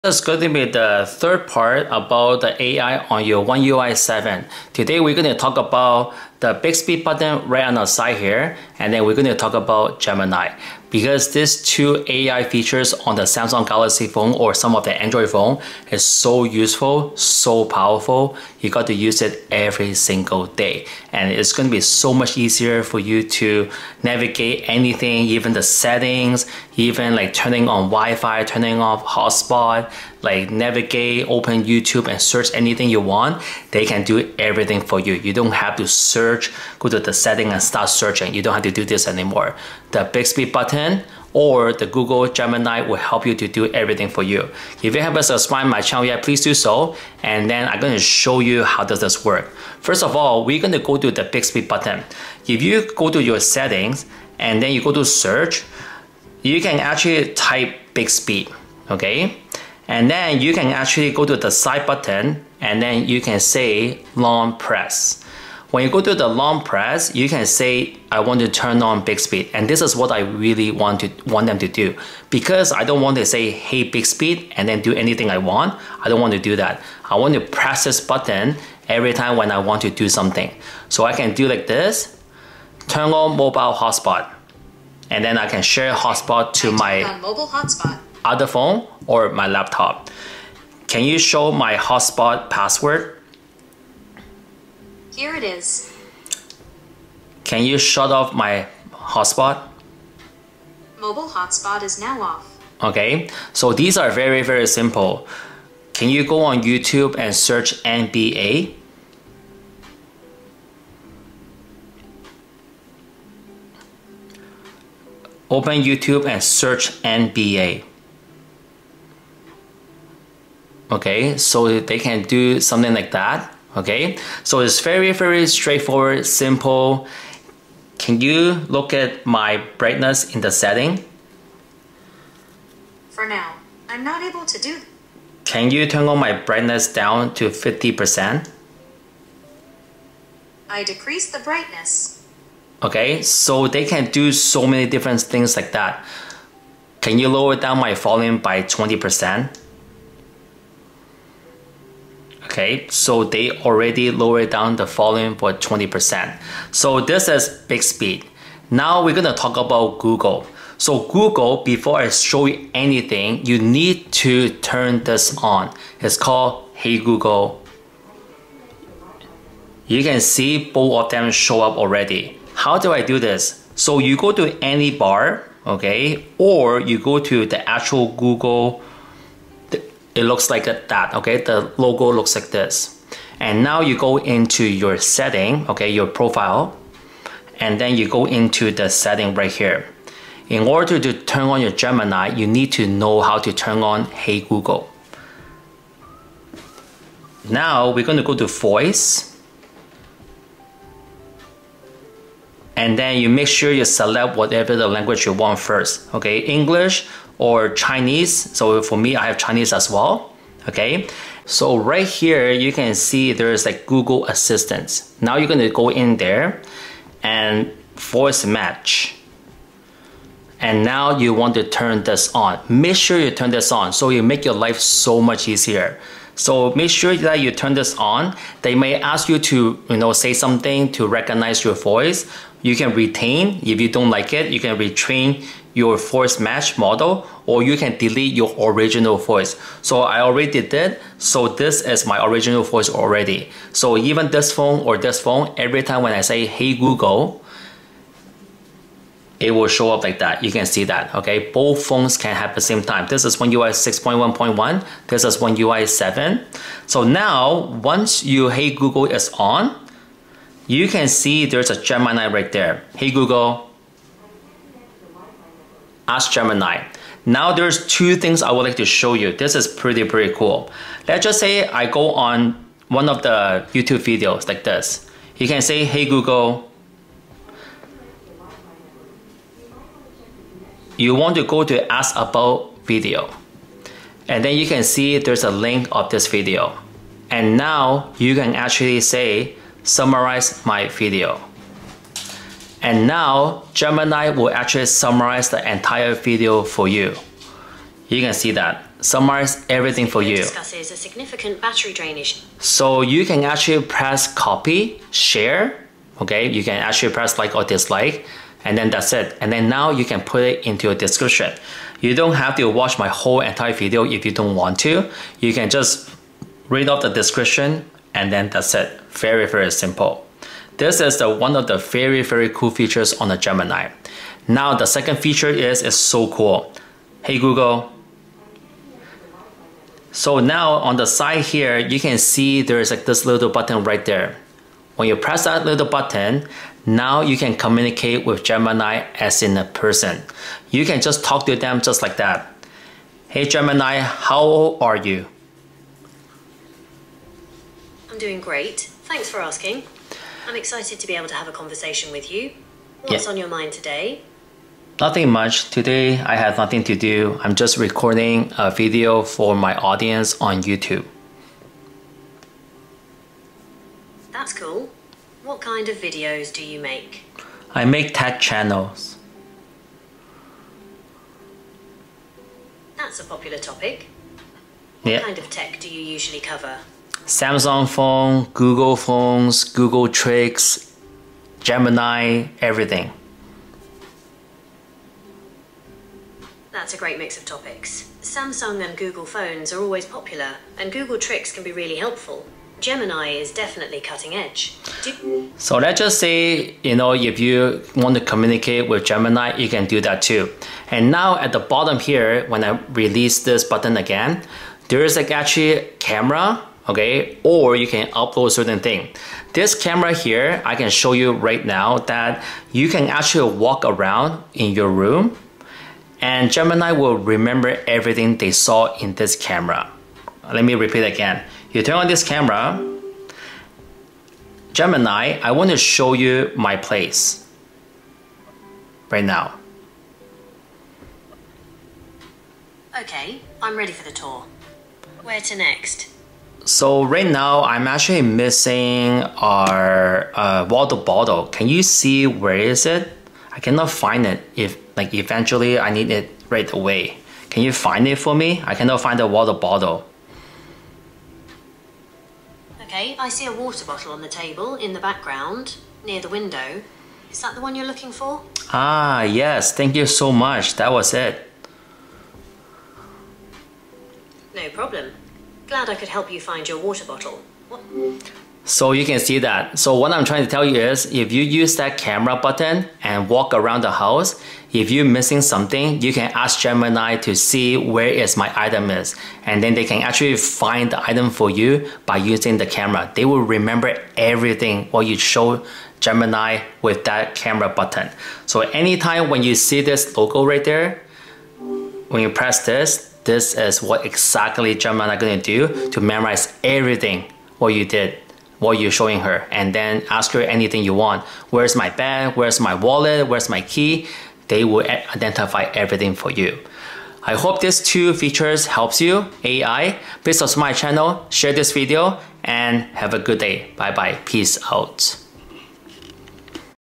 2 Powerful Samsung Galaxy Features I Can't Live Without!!!
Here's a summary of the video:

This video highlights two powerful AI features on Samsung Galaxy phones: the Bixby button and Google Gemini.

Bixby Button:
 * The user demonstrates how to customize the side button (Bixby button) for quick actions using a long press

Google Gemini:
 * The video explains how to enable "Hey Google" in the Google app's settings to access Gemini
 * Two key Gemini feature showcased:
   * Video Summarization: While watching a YouTube video, you can say "Hey Google, ask Gemini to summarize my video" to get a quick summary of the content

   * Interactive Communication with Camera Input: By pressing a button within Gemini, you can talk to it conversationally and even use the camera to show it your surroundings. Gemini can then recall what it has "seen" and answer questions about it, such as locating a missing water bottle
In conclusion, the video emphasizes the convenience and power of using the Bixby button for quick actions and Google Gemini for tasks like video summarization and identifying objects through the phone's camera